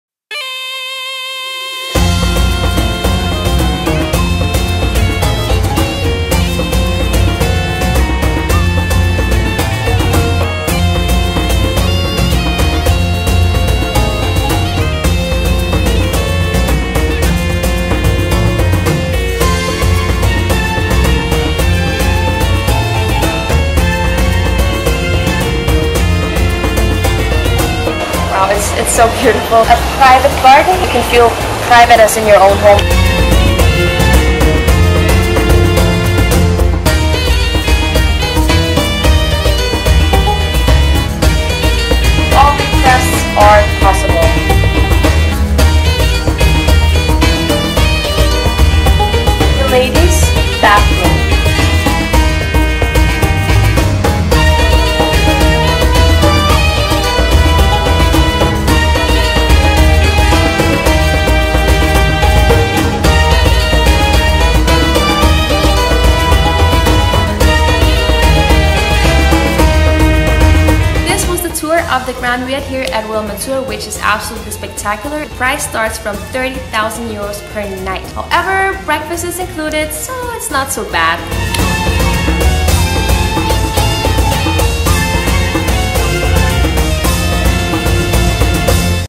THE + 30.000 €/NIGHT Grand Riad at Royal Mansour - show-around by InspectorLUX
InspectorLUX gives you exclusive footage of the most expensive Riad at hotel Royal Mansour in Marrakech. Celebrities & the king, who also commissioned the hotel, stay in this riad.